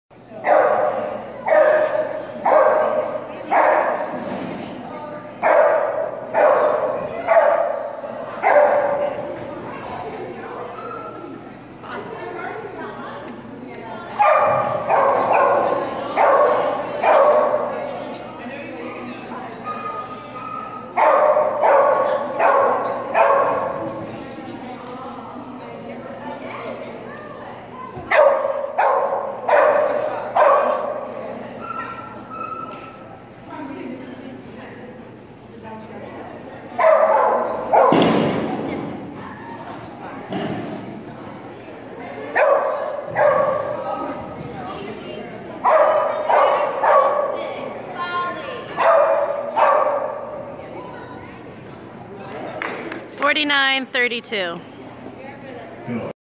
Finn's agility Exc Std
Dec.16, 2016, Exc. Std. Preferred, 20". Great run, got his weaves, just the table springboard move...! Where is this coming from!